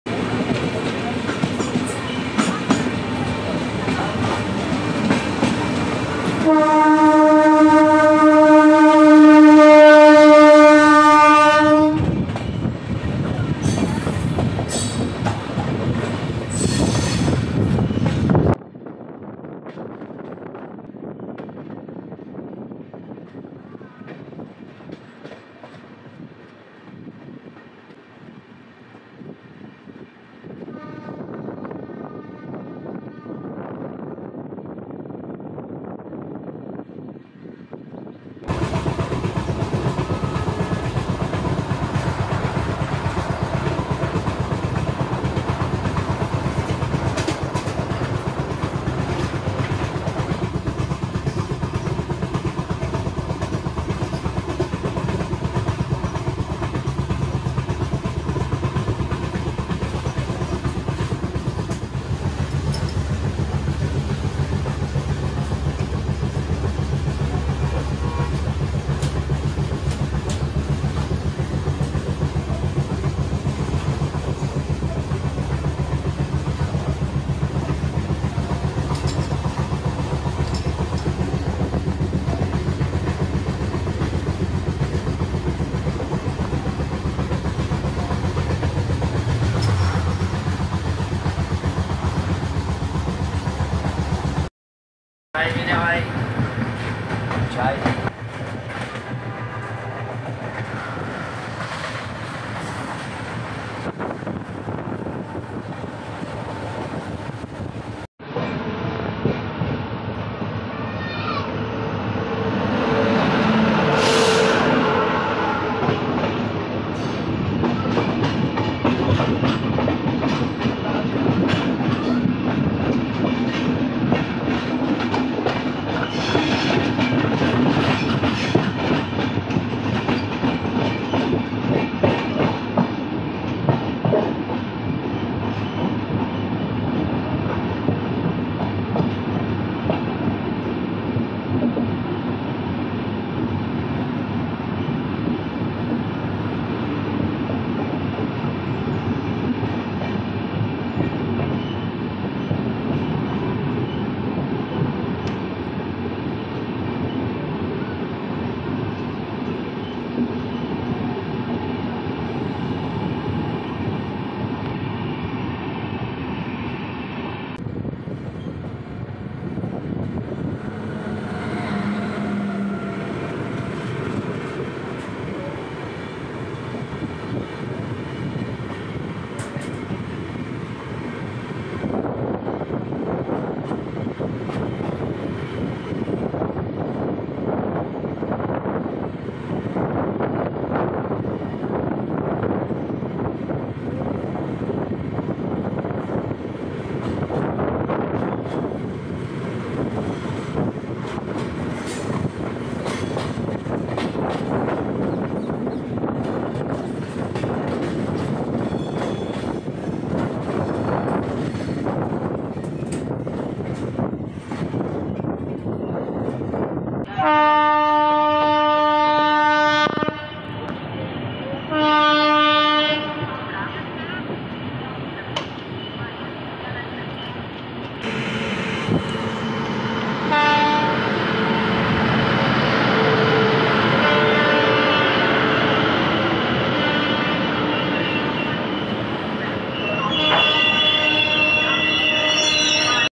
10 Back to Back Train actions captured through the window of running train | Parallel Run and Cross
captured multiple trains from the window of Paschim express on amritsar Ludhiana route.

Some trains include-

Vaishno devi Vande Bharat express
Subedarganj Express
Hisar Amritsar express
Ludhiana Amritsar MEMU
Jalandhar Pathankot DMU
Lohian Ludhiana DMU
Chandigarh Amritsar express